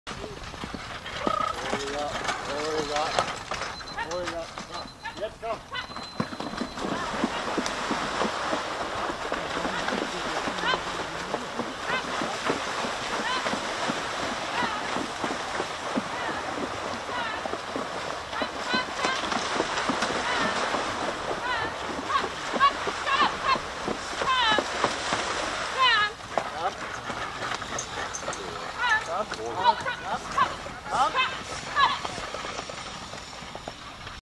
Fahrturnier Lienen 4.6.2023
Ein kurzes Video einer Marathon-Fahr-Prüfung vom Fahrturnier in Lienen